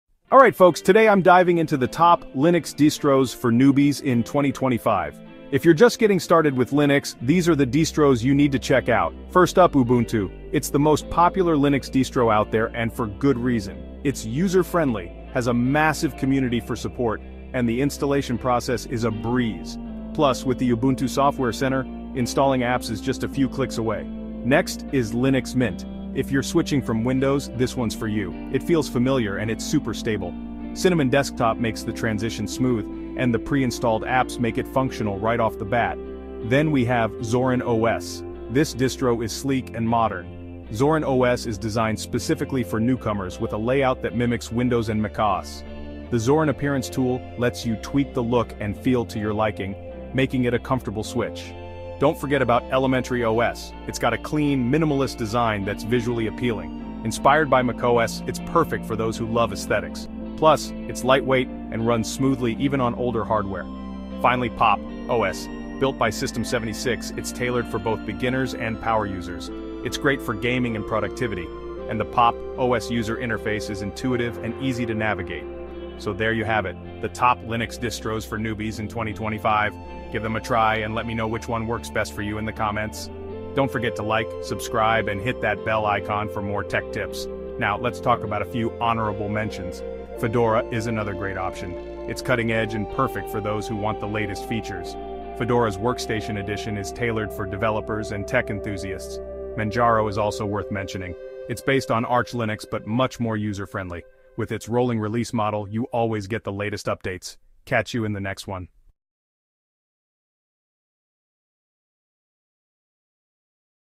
Best Linux Distros for Beginners in 2025!
Are you new to the world of Linux and unsure which distro to start with? In this video, we explore the **best Linux distros for beginners in 2025**! Whether you're looking for user-friendly interfaces, extensive community support, or a smooth learning curve, we've got you covered. Join us as we break down the top contenders, including **Ubuntu, Linux Mint, and Zorin OS**, highlighting their unique features and advantages. By the end of this video, you'll have a clearer understanding of which Linux distribution suits your needs best.